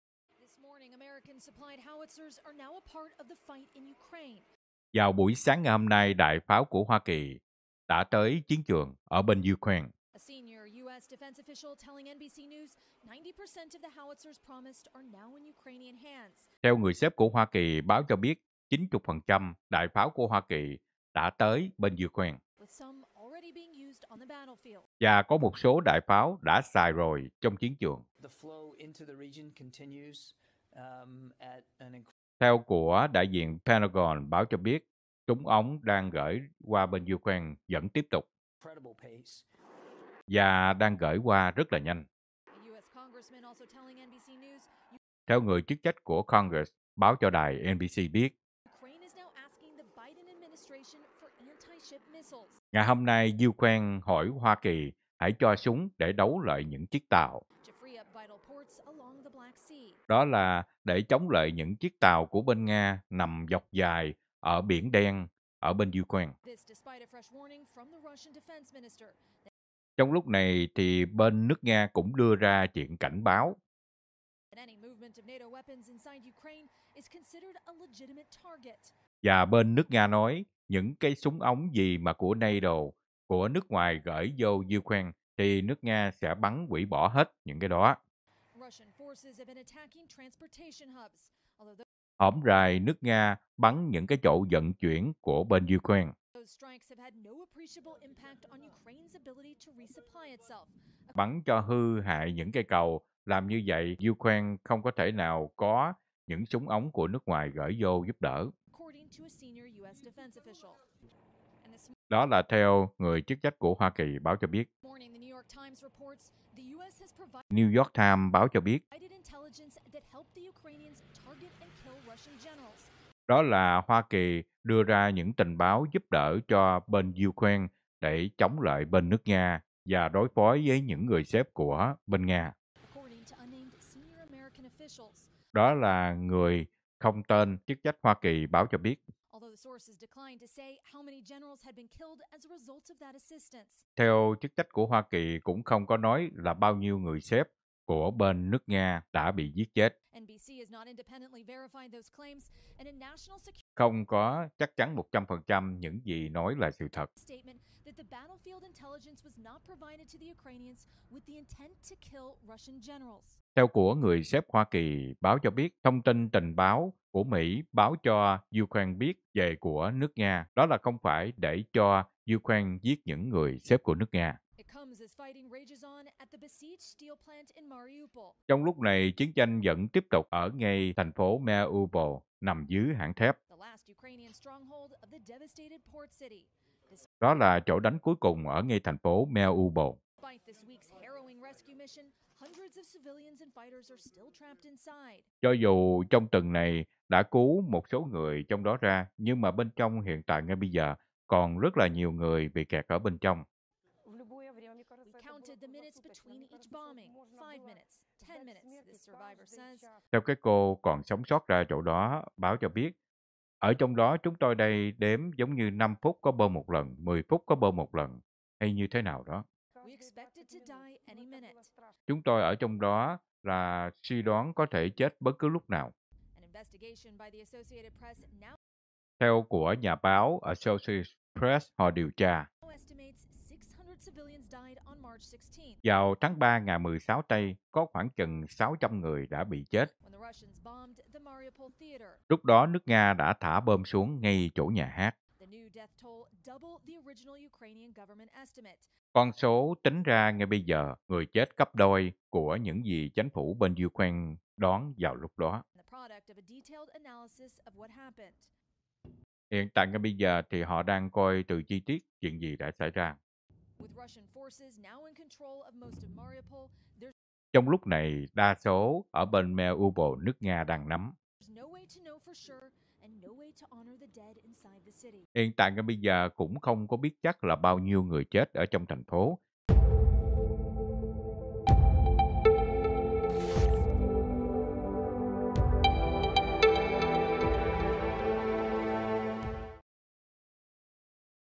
90% đại pháo CỦA HOA KỲ ĐÃ ĐẾN ukraine | UNV Tin Tức 7/5/2022
90% đại pháo của hoa kỳ đã đến ukraine.  UNV Tin Tức 7/5/2022